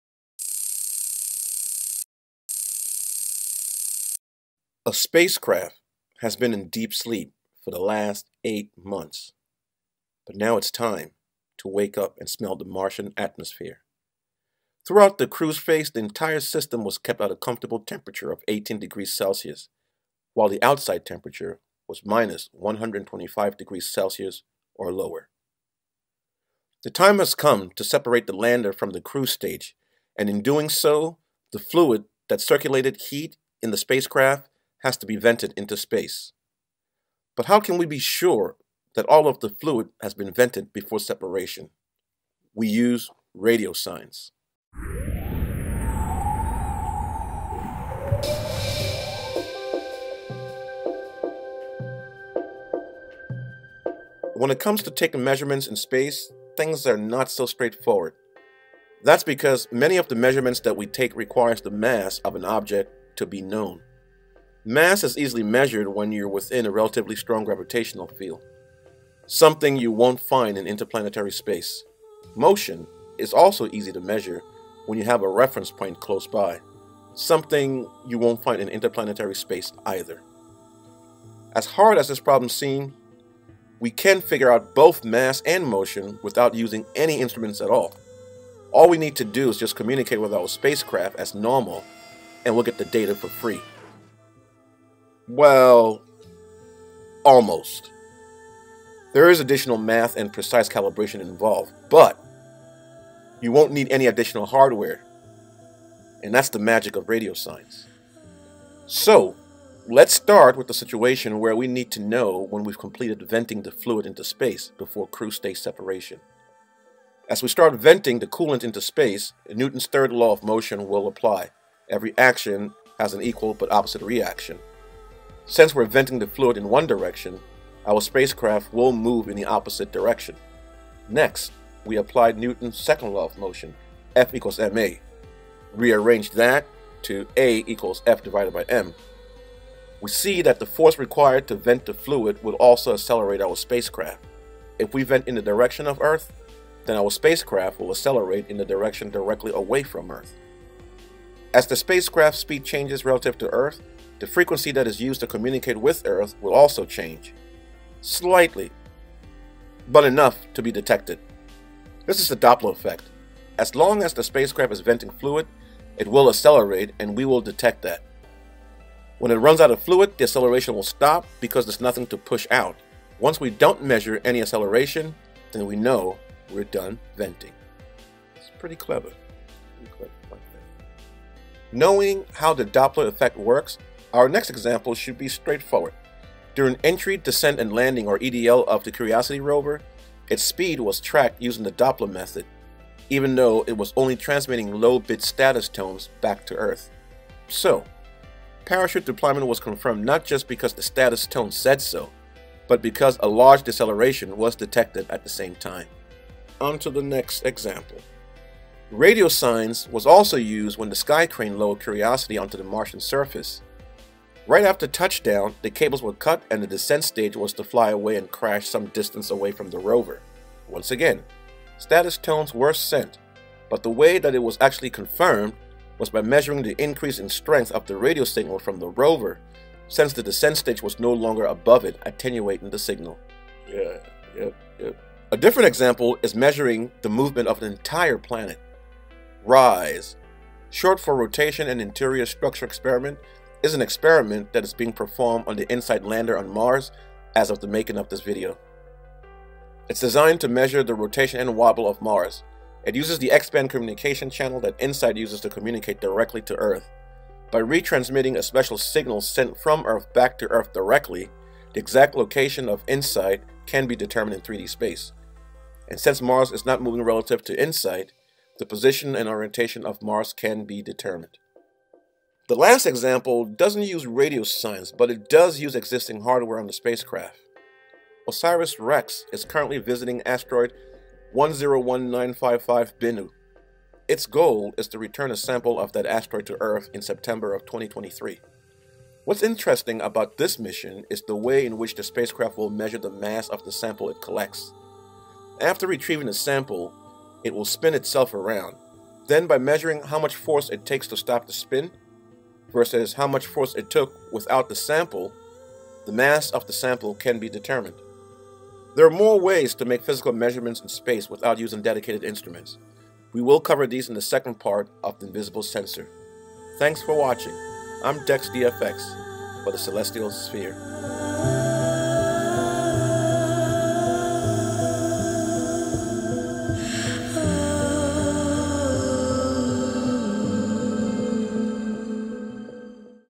*Radio Science - part 1
Radio waves are used to communicated with spacecraft. We use them to send commands to the spacecraft and download data from them. But in many cases we can use the radio wave itself as a measuring instrument or sensor. In this episode of Celestial Sphere we look at few examples where radio waves are used as part of the sensors suite on a spacecraft. This is a two part series. Part 2 will focus on the use of radio waves to make scientific observations and take measurements .